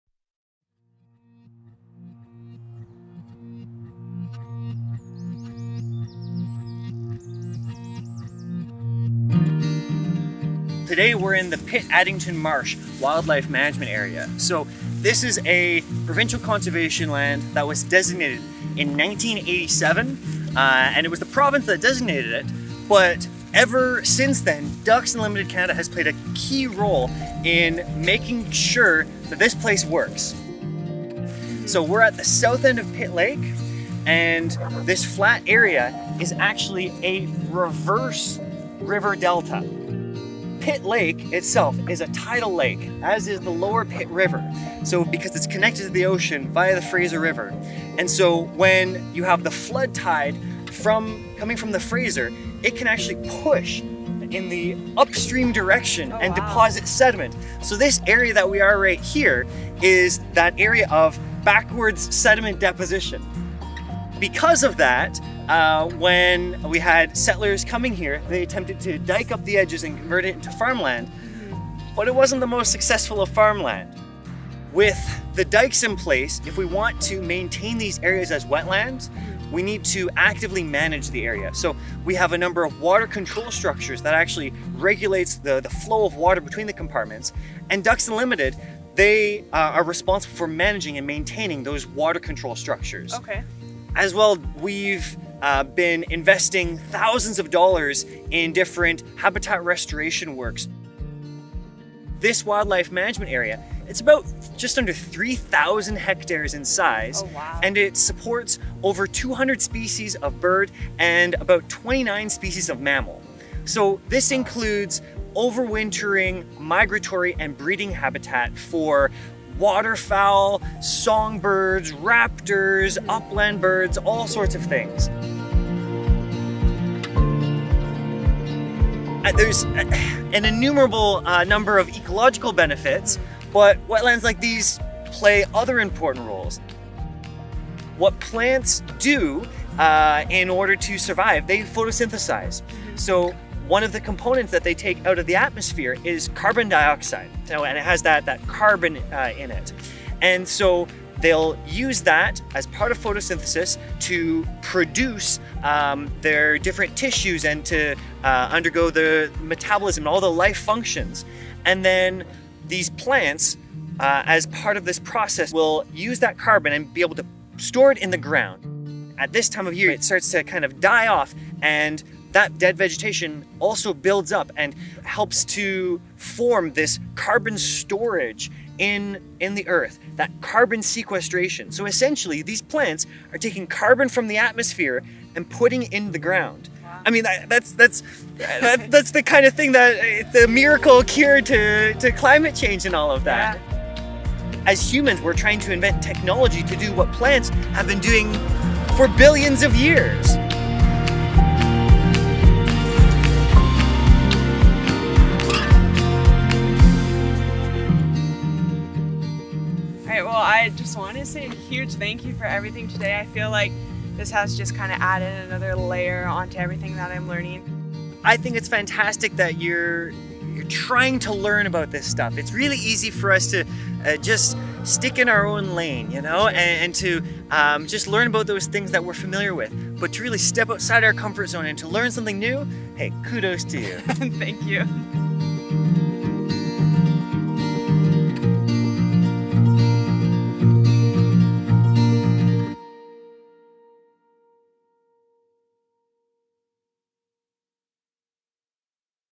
Pitt Addington Marsh
Experience Pitt Addington Marsh Wildlife Management Area, one of Canada's most beautiful landscapes and the filming location for Under the Wing.